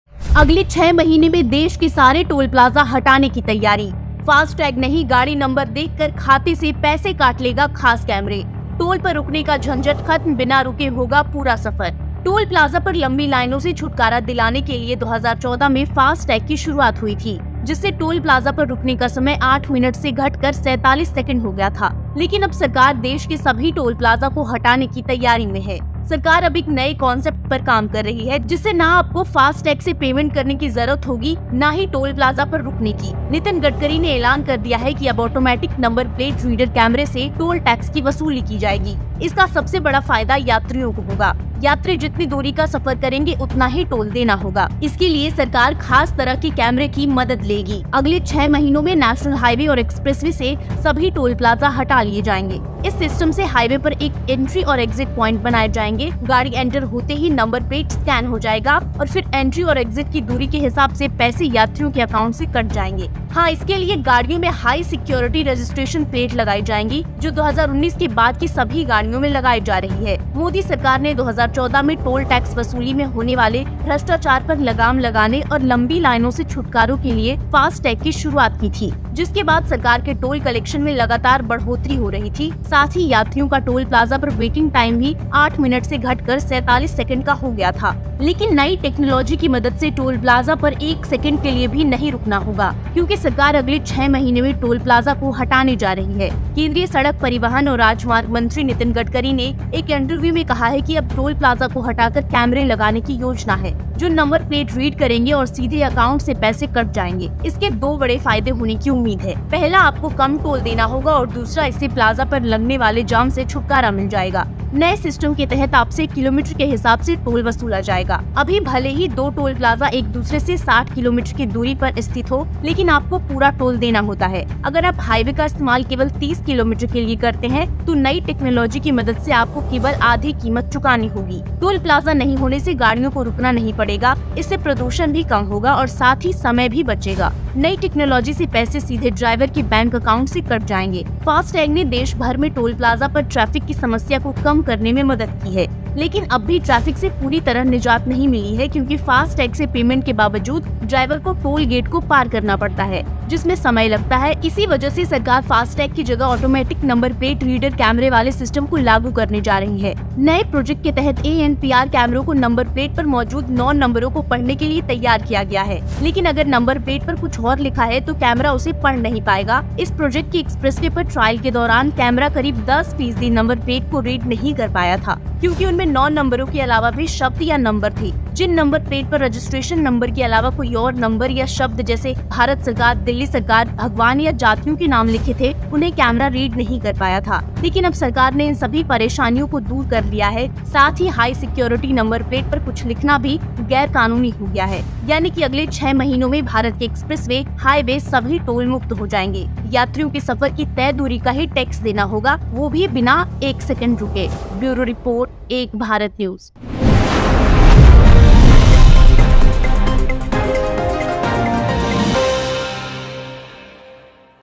No more FasTag! Center plan to remove all the toll plaza in the country
टोल प्लाजा पर लंबी लाइनों से छुटकारा दिलाने के लिए 2014 में फास्टैग की शुरूआत हुई थी.. जिससे टोल प्लाजा पर रुकने का समय 8 मिनट से घटकर 47 सेकेंड हो गया था.. लेकिन अब सरकार देश के सभी टोल प्लाजा को हटाने की तैयारी में है.. सरकार अब एक नए कॉन्सेप्ट पर काम कर रही है..जिससे न आपको FASTag  से पेमेंट करने की जरुरत होगी..न ही टोल प्लाजा पर रुकने की.. नितिन गडकरी ने ऐलान कर दिया है कि अब ऑटोमेटिक नंबर प्लेट रीडर कैमरे से टोल टैक्स की वसूली की जाएगी.. इसका सबसे बड़ा फायदा यात्रियों को होगा.. यात्री जितनी दूरी का सफर करेंगे उतना ही टोल देना होगा.. इसके लिए सरकार खास तरह के कैमरे की मदद लेगी.. अगले 6 महीनों में नेशनल हाईवे और एक्सप्रेसवे से सभी टोल प्लाजा हटाए जाएंगे.. इस सिस्टम में हाईवे पर एक एंट्री और एग्जिट पॉइंट बनाए जाएंगे. गाड़ी एंटर होते ही नंबर प्लेट स्कैन हो जाएगा. फिर एंट्री और एग्जिट की दूरी के हिसाब से पैसे यात्रियों के अकाउंट से कट जाएंगे।